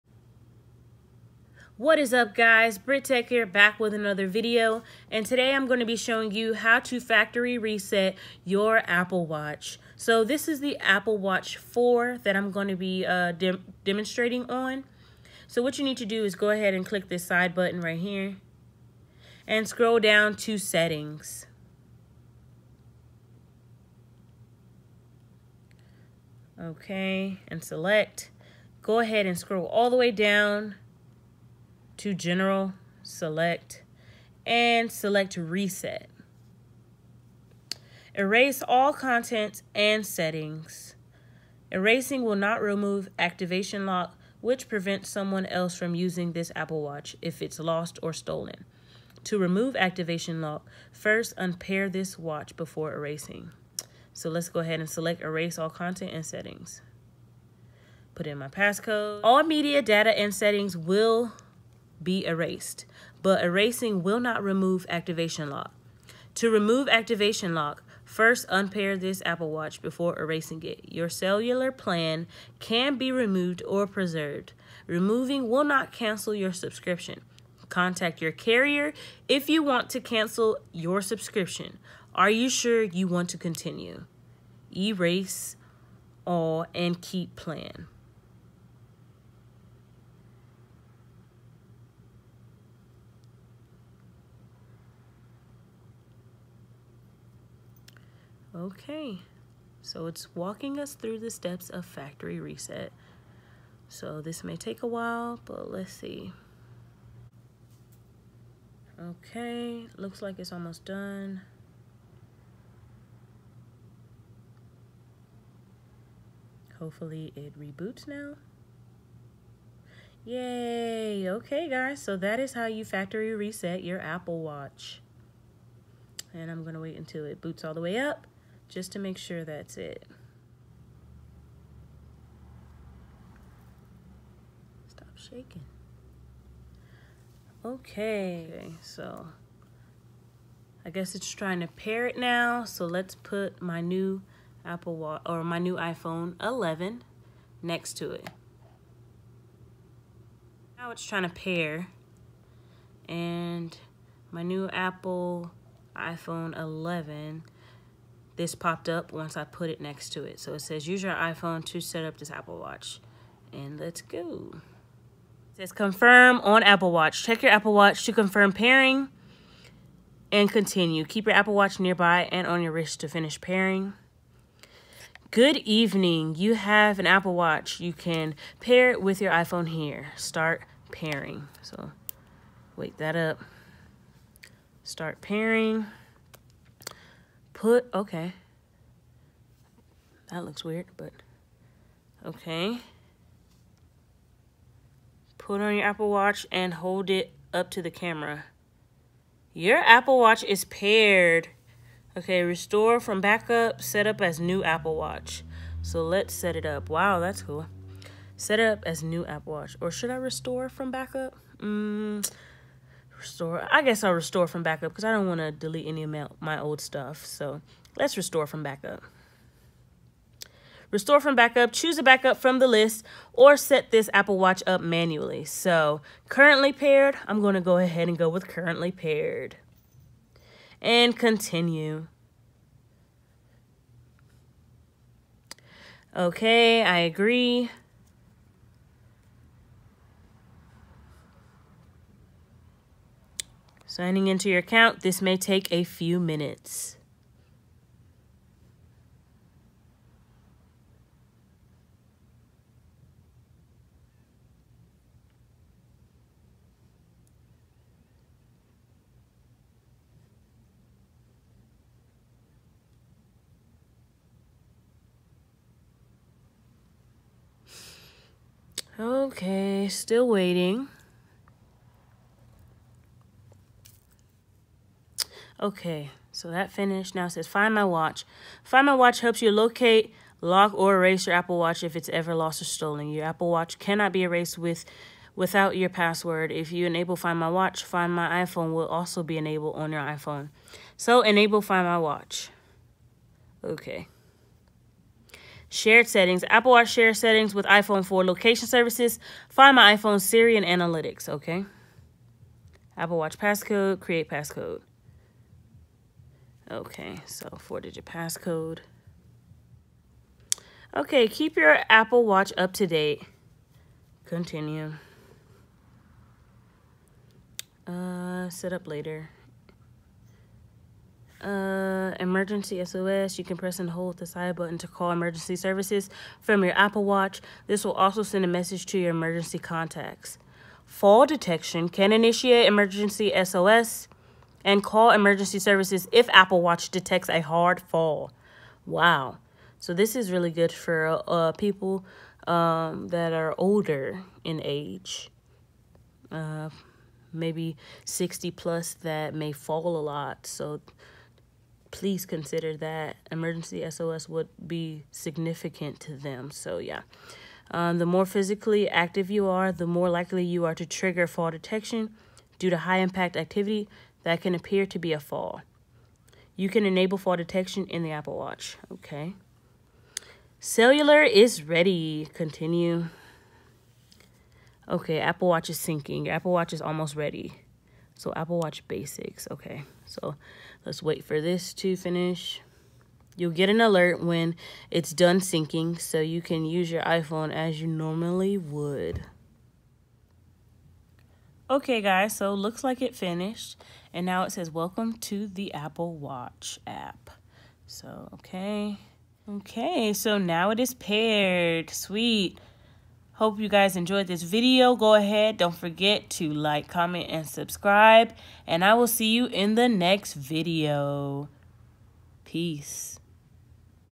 How To Factory Reset Apple Watch And Pair With IPhone
In this video I show you how to factory reset your Apple Watch and pair it with a new iPhone.